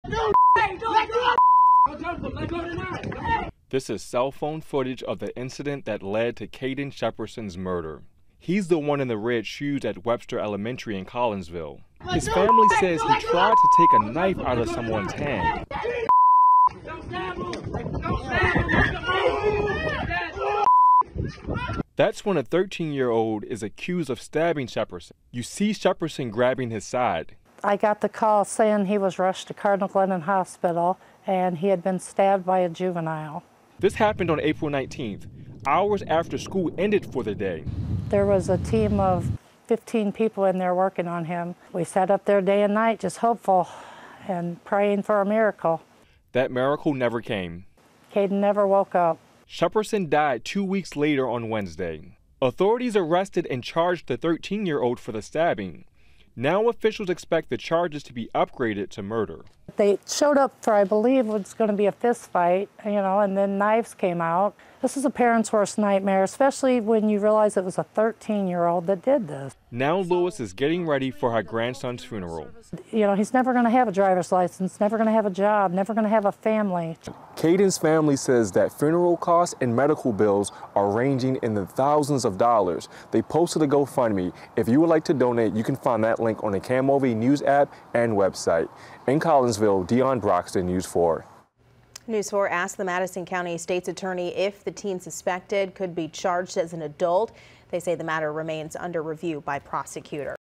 2 Metro East juveniles in custody for stabbing death of 16-year-old
2 Metro East juveniles in custody for stabbing death of 16-year-old